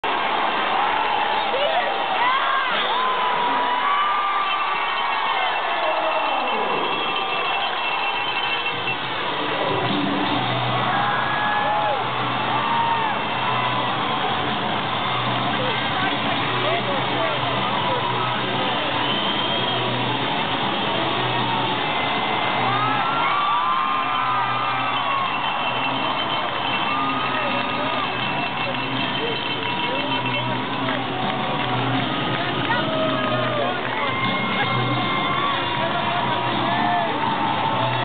Jason Kostadine Vs. Joe Grimaldi
Jason Kostadine, of the Quad City Mallards, fights Joe Grimaldi, of the Rapid City Rush